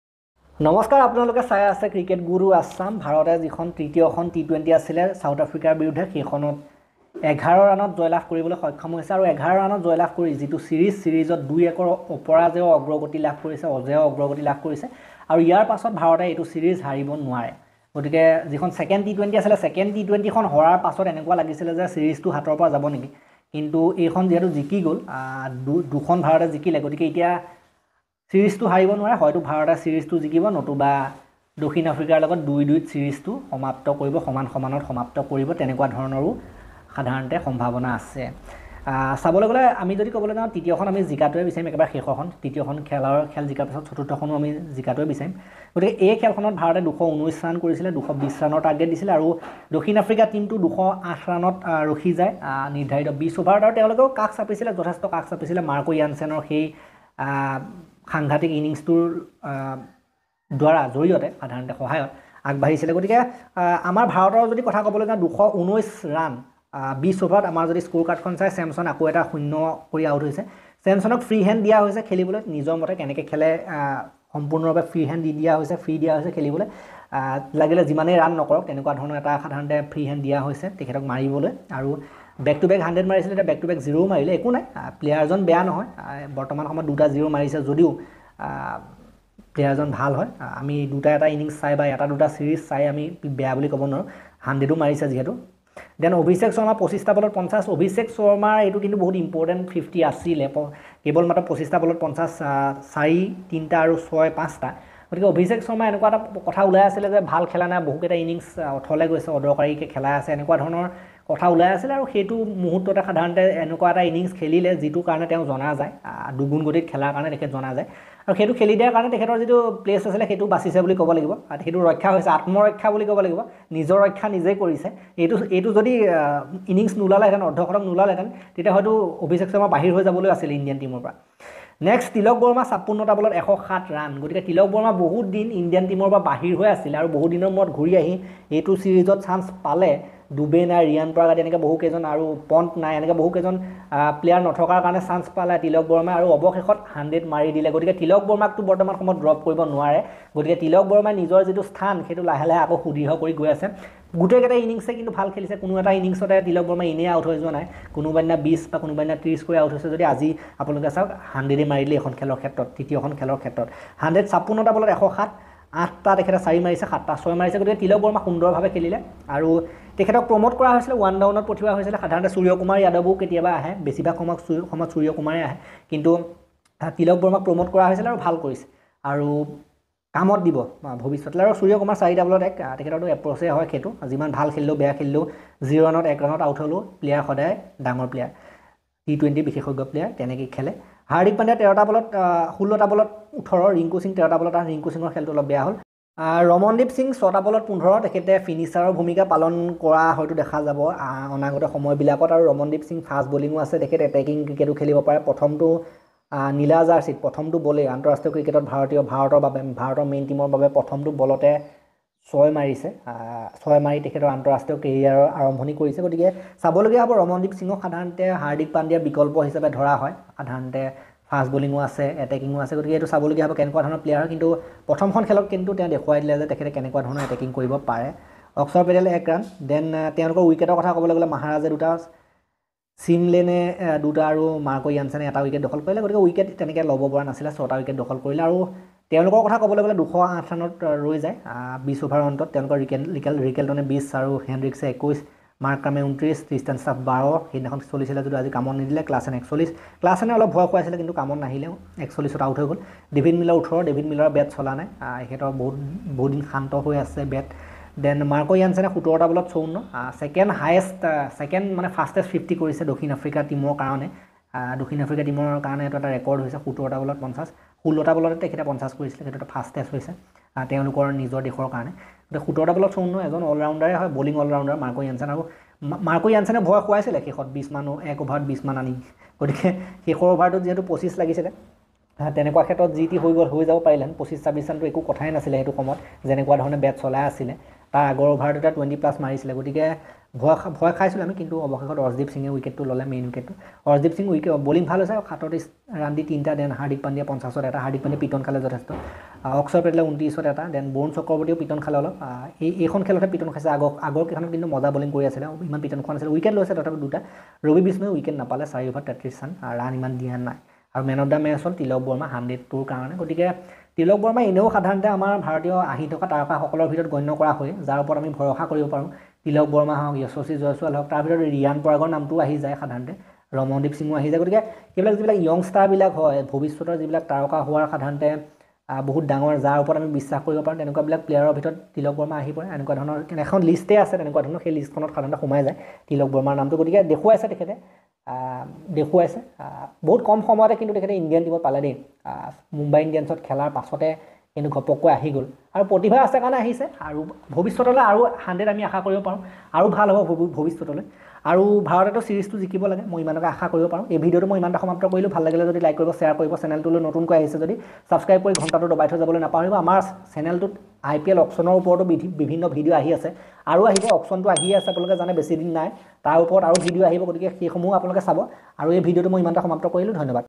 India Beat South Africa & Lead 2-1 in Series | Tilak Varma Brilliant 100
India Beat South Africa in 3rd T20I